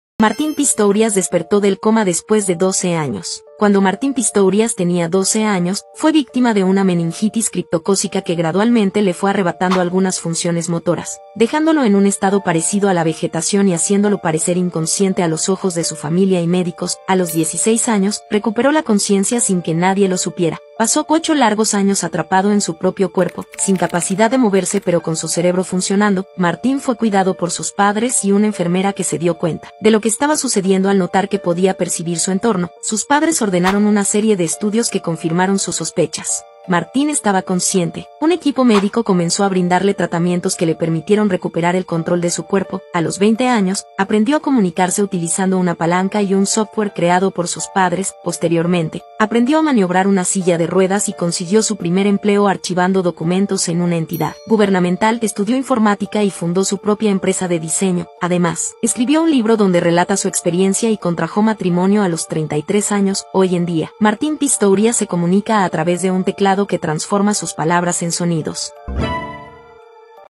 Martin Pistorius despierta después de 12 años
En este asombroso relato, descubrimos la increíble historia de Martin Pistorius, quien despertó después de estar en estado de coma durante 12 años. A través de su determinación y del apoyo de sus seres queridos, logró superar todas las adversidades y recuperar su voz y movilidad. En este video, exploraremos los detalles de su experiencia y reflexionaremos sobre el poder de la resiliencia humana. ¡No te pierdas esta inspiradora historia de esperanza y superación!